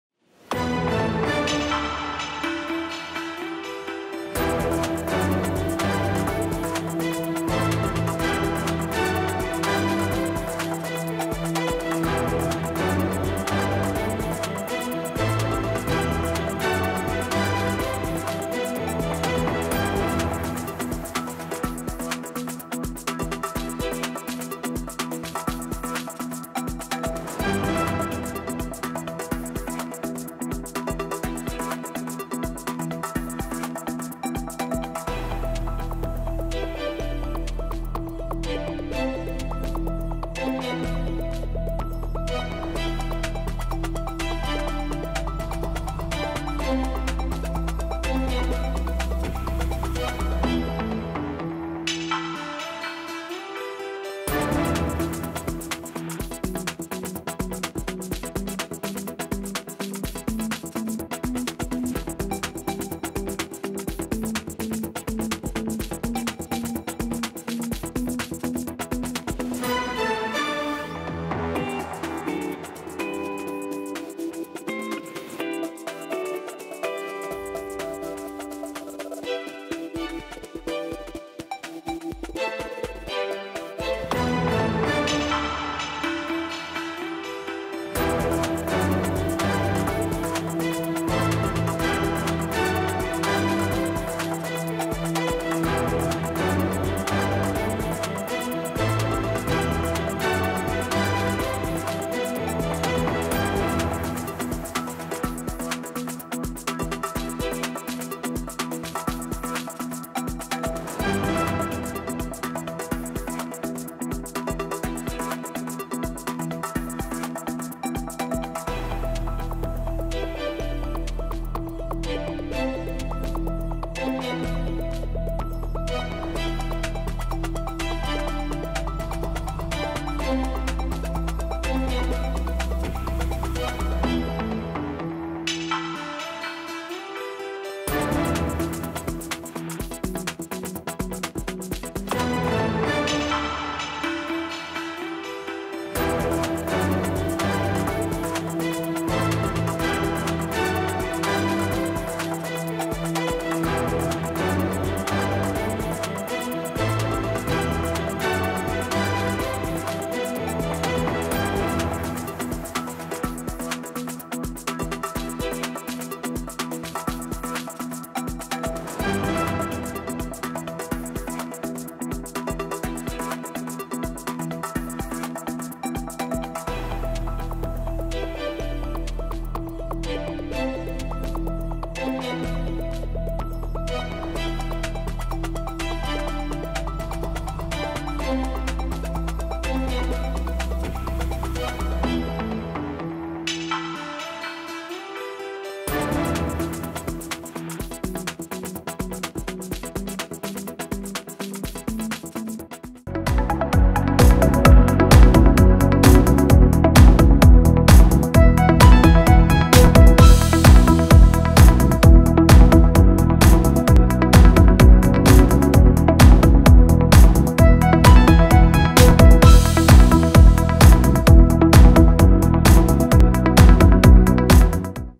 Madhya Pradesh Lok Sabha Election Result 2024: लोकसभा चुनाव में किसको जनता ने नकार दिया ? MP Tak
Madhya Pradesh की सभी 29 की 29 सीटों पर BJP के यह सारे प्रत्याशी हार गए ,Madhya Pradesh में BJP का क्लीन स्वीप, आखिरकार MP में चल गई मोदी की गारंटी।